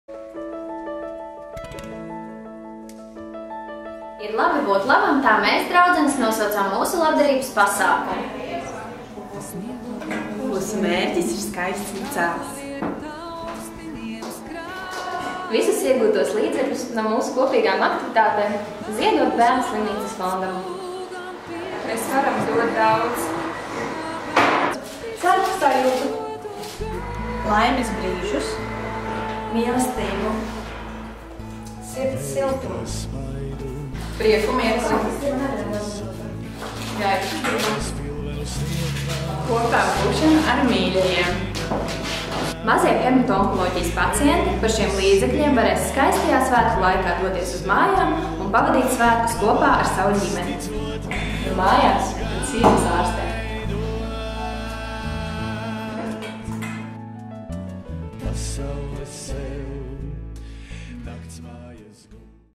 Ir Labi Būt Labam
"Ir labi būt labam" ir par skaistu kopā būšanu, tāpēc mēs - jaunas, skaistas, labsirdīgas - sanākam kopā, lai apgleznotu legingus, uzceptu kādu gardumu vai veiktu kādu citu kopīgu aktivitāti. Mūsu moto - “Mēs iegūstam dodot”.
Pagājušajā gadā apgleznojām 13 legingus, kurus izsolījām mūsu Facebook lapā “Ir labi būt labam”. Izsolē iegūtos 433,00 EUR ziedojām Bērnu slimnīcas fondam “Braucam mājās”. Mazie hematoonkoloģijas pacienti par šiem līdzekļiem skaistajā svētku laikā varēja nokļūt mājās un svētkus pavadīt kopā ar ģimeni. Bērnu slimnīcas hematoonkoloģijas nodaļā ir tādi bērni, kuri pazeminātās imunitātes dēļ nevar izmantot sabiedrisko transportu.

Šogad esam vienojušās jaunā, skaistā aktivitātē - cepām makarūnus brīnišķīgās Annas Birmanes meistarklasē. Sākot no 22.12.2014 tos var iegādāties mūsu Facebook lapā. Visi iegūtie līdzekļi tiks ziedoti Bērnu slimnīcas fondam “Braucam mājās”.
Mēs svētku laikā pērkam kādu gardumu svētku galdam vai dāvanu tuvam cilvēkam, bet varbūt, to darot, varam ziedot tiem, kam to patiešām vajag.

Mēs priecāsimies, ja mūsu stāsts radīs vēlmi palīdzēt, ja ne mūsu projektam, tad varbūt kādam citam labam darbam.

Paldies par atsaucību tiem, kas atbalsta mūsu ideju!